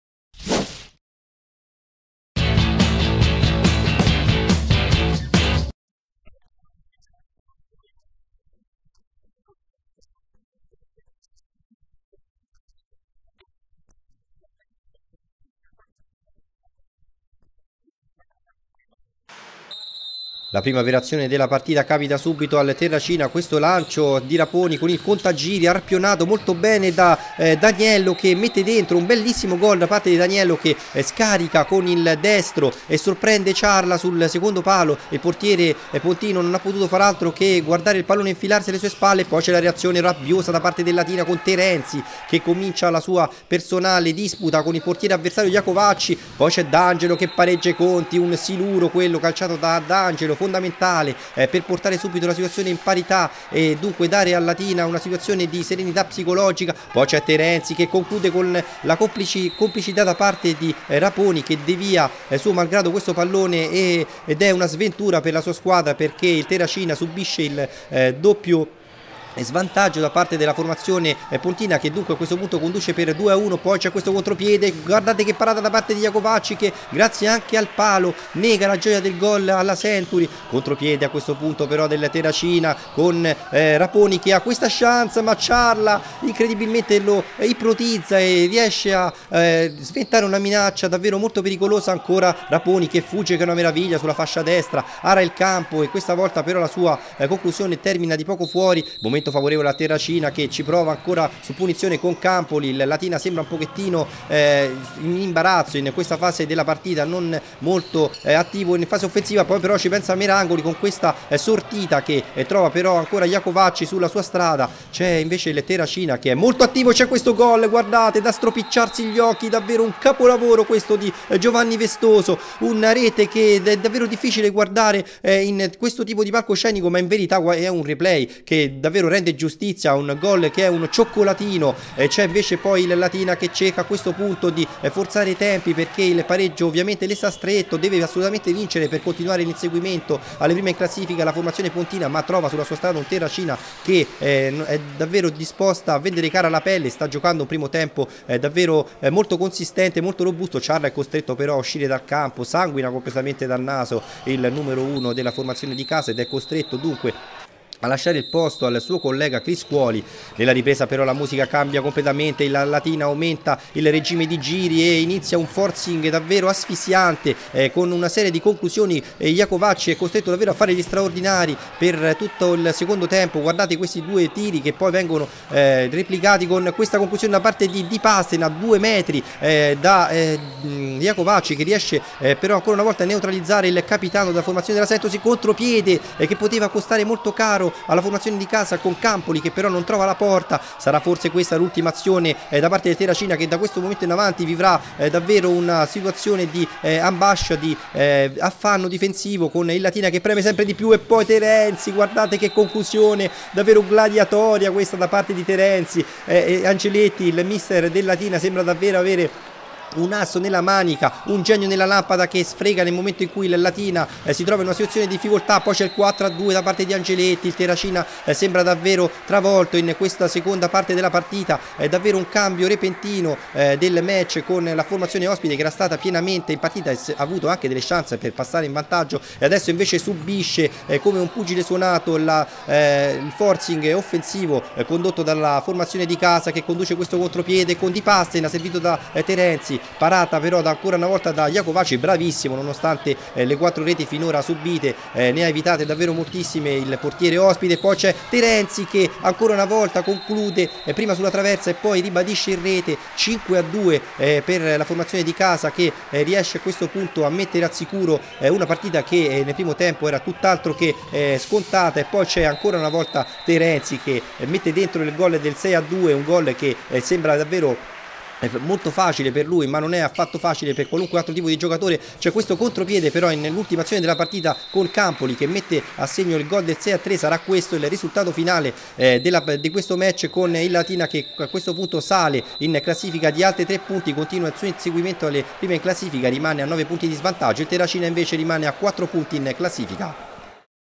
Futsal Latina vs Terracina Calcio a 5: Highlights
FUTSAL LATINA: Ciarla, Abbenda, Di Pastena, D'Angelo, Merangoli, Martufi, Gaito, Terenzi, Corbi, Angeletti, Bulboaca, Criscuoli. All. Angeletti.
TERRACINA CALCIO A 5: Giovampietro, Raponi, Pestillo, Campoli, Pernarella, Avagliano, Pellino, G. Vestoso, D'Aniello,  R. Vestoso,  S. Pellino, Iacovacci. All. Cardillo.
ARBITRO: Danilo di Roma due.
MARCATORI: 2' D'Aniello, 3' D'Angelo, 6' Terenzi, 27'G.Vestoso, 6'st Terenzi, 11'st Angeletti, 29'st Terenzi, 30'st Terenzi, 30'st Campoli.
NOTE: recupero: 2' pt, 1'st. Ammonito A.Pellino.